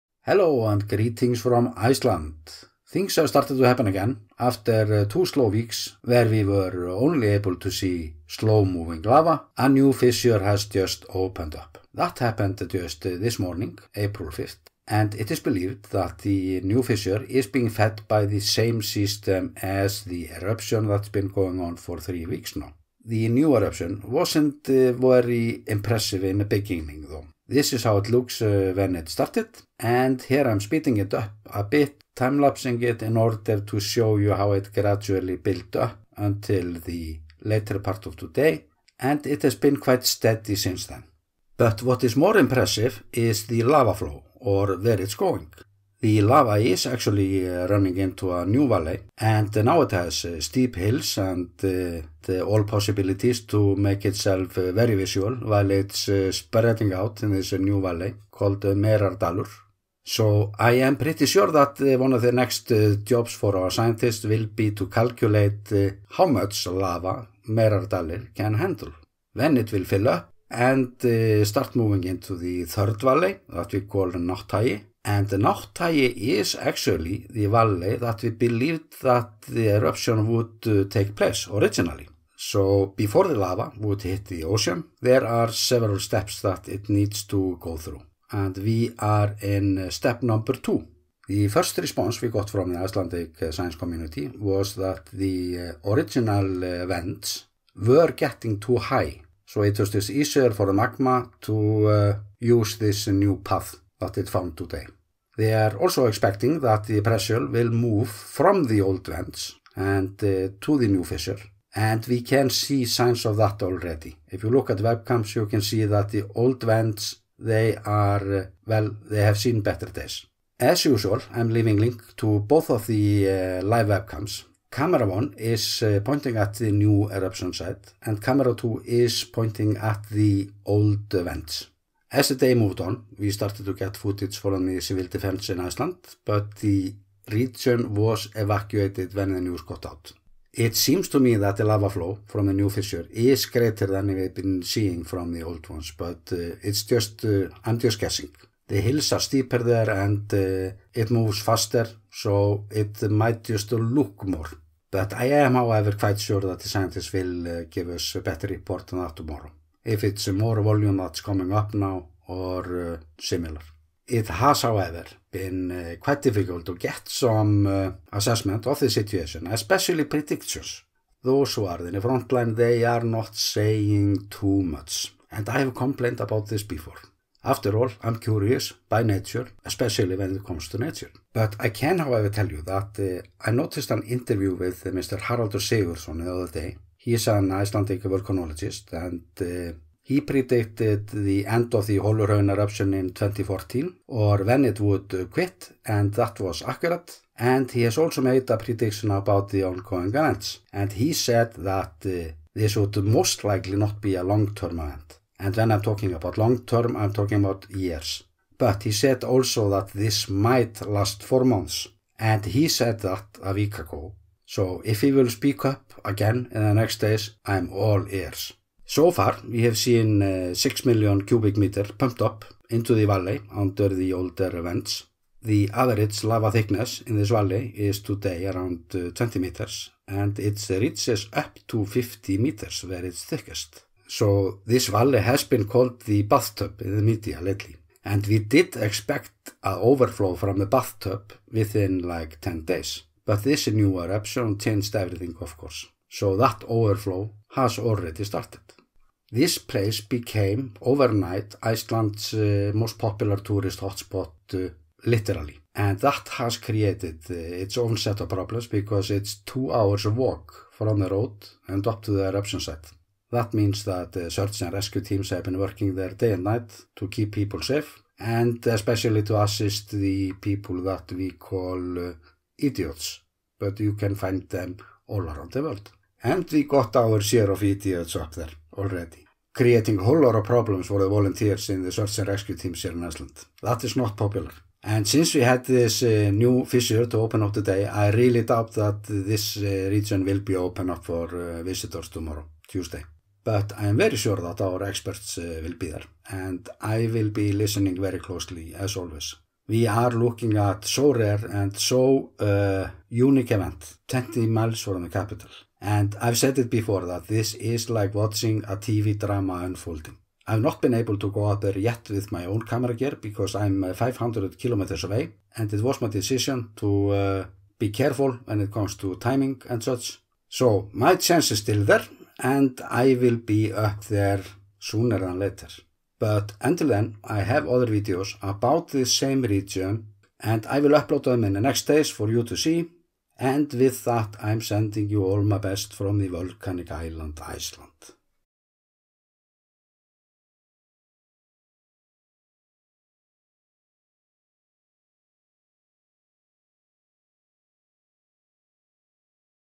New Fissure in the Geldingadalir Volcano in Iceland - News Update
Latest news and footage from the Icelandic volcano who decided to open up a new fissure today, April 5th. The photos are from the Icelandic Civil protection Agency and the video is from RUV - The Icelandic National Broadcasting Service. Links to their live cam is to be found here below:

Earthquakes on aerial photo layer.

Do you want to own high quality prints of Iceland, Icelandic nature and volcanoes?